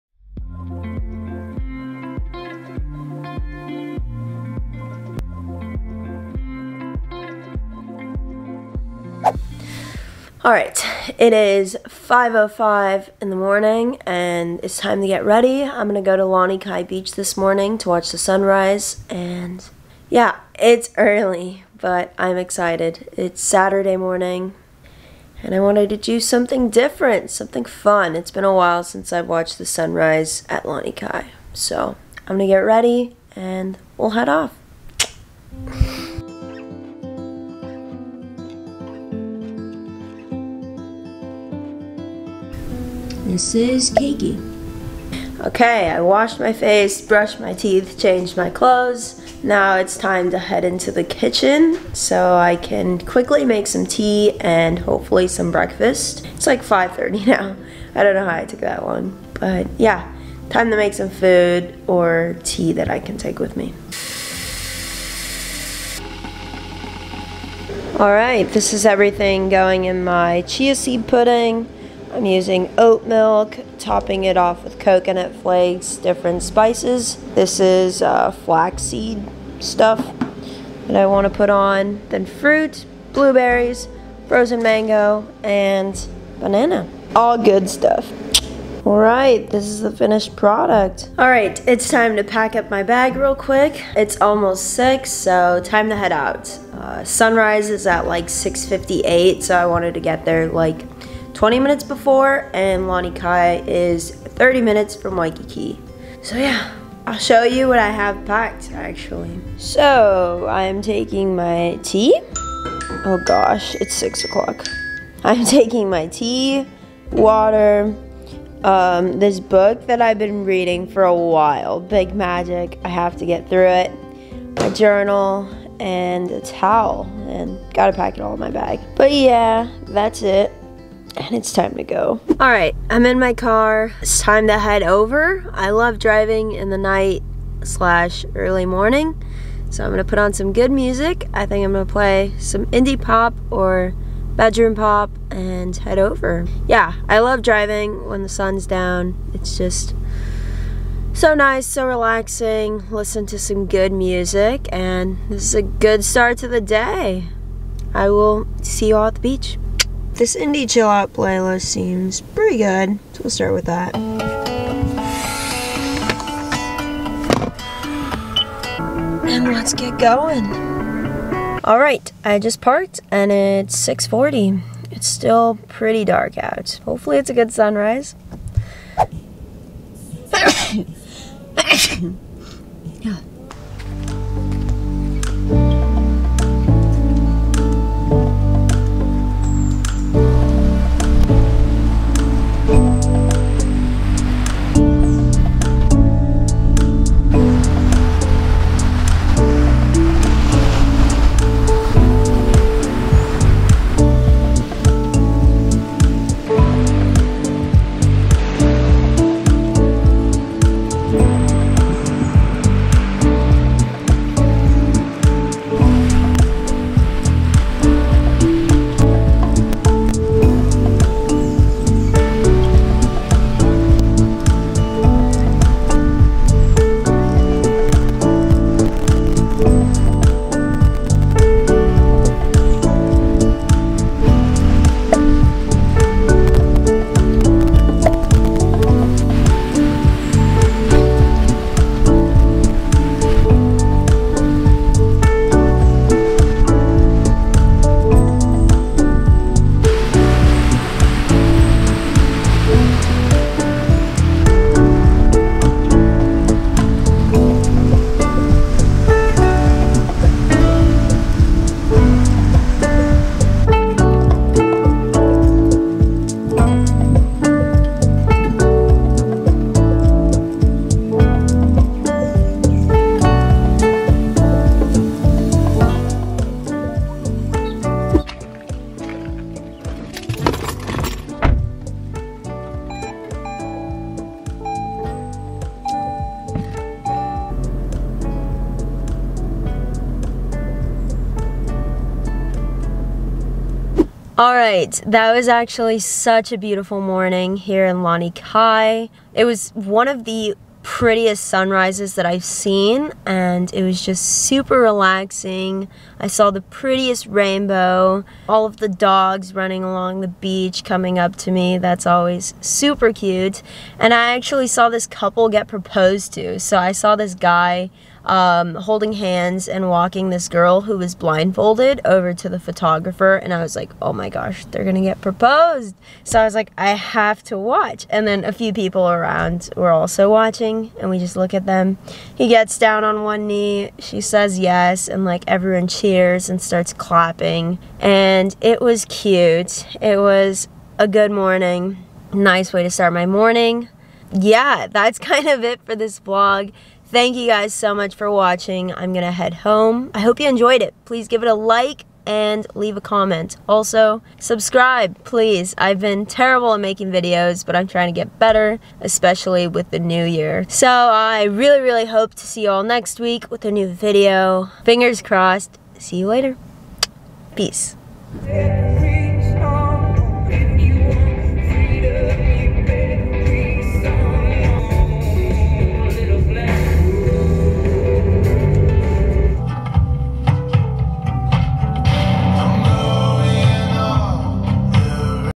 Relaxing Morning in Honolulu, Hawaii Vlog (Beautiful Sunrise, Solo Beach Trip, Lanikai Beach)
Life in Oahu, Hawaii--A slow, relaxing morning in the life of PhD student living in Honolulu. Hawaii Vlog 2022.

Hi all! My name is Alexus Cazares and I am a second year PhD/grad student studying Biological Oceanography at UH Manoa in Honolulu, Hawaii on the island of Oahu.

In this week's vlog, I wake up at 5 am to take you to one of my favorite places to watch the sunrise.. Lanikai Beach in east Honolulu! I show you how I get ready for a solo beach trip, a little bit around Lanikai Beach, a sunrise proposal, cute dogs, the Mokes, a beautiful rainbow, and more!

Thank you all so much for watching and I hope you enjoyed it!